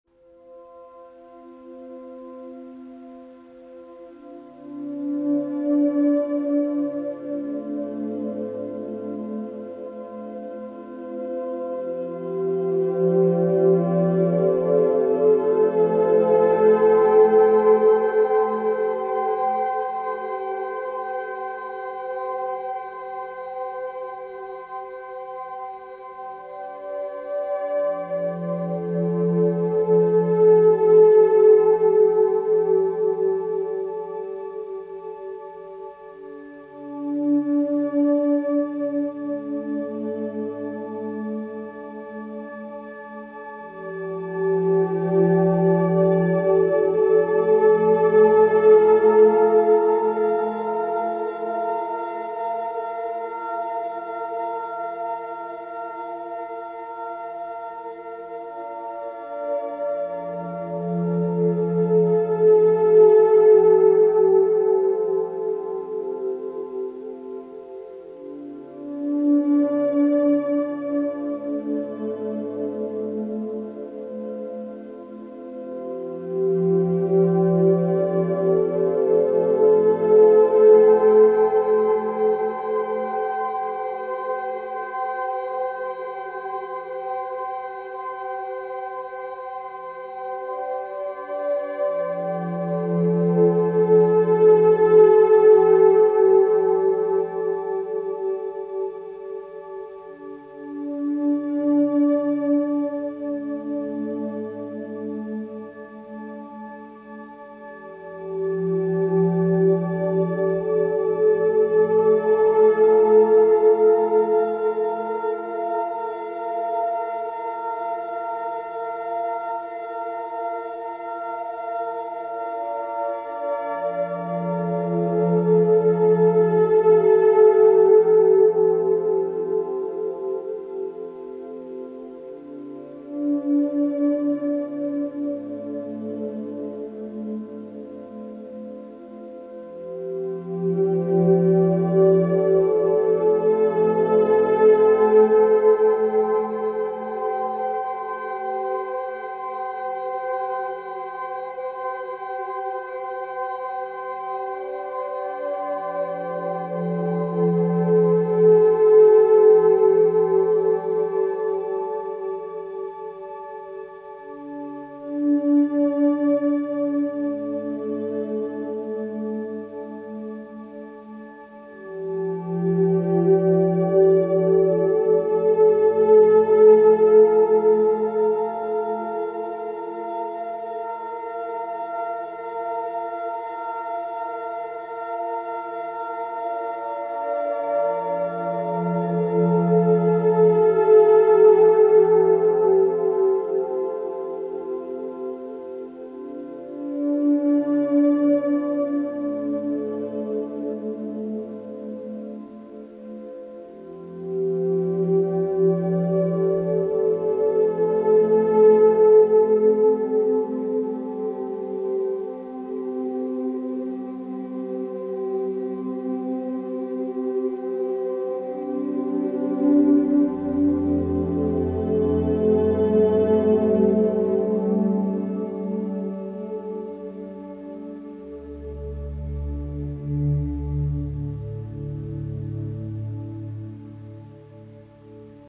Nasa "ISV Columbus" - Marte missione segreta (1973)
ISV Columbus (1973), missione segreta della Nasa sul pianeta rosso Marte!